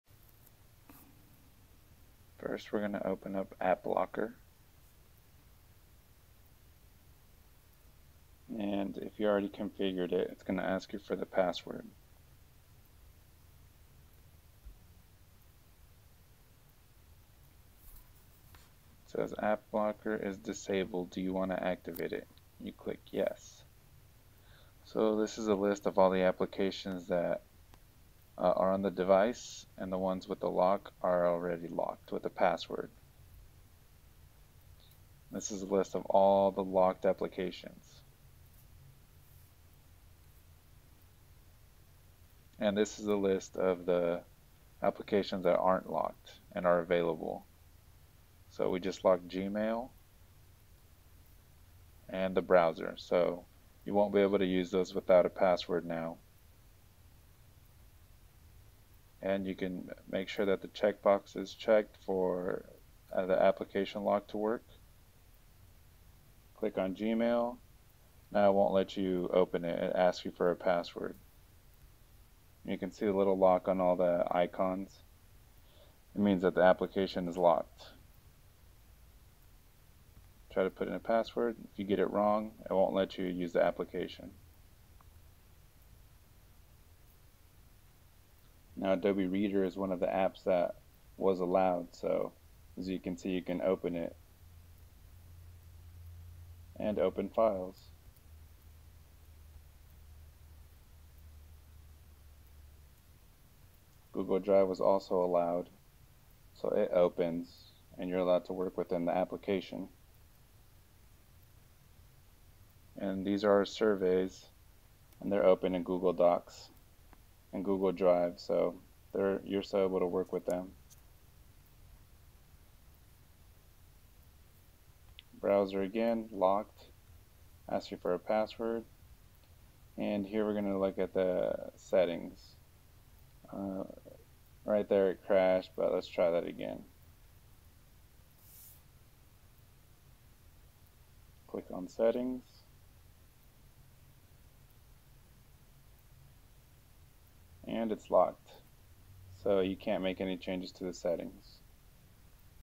App Lock Demo On Asus Transformer
Demo of App Lock on the Asus Transformer showing how it restricts applications and settings.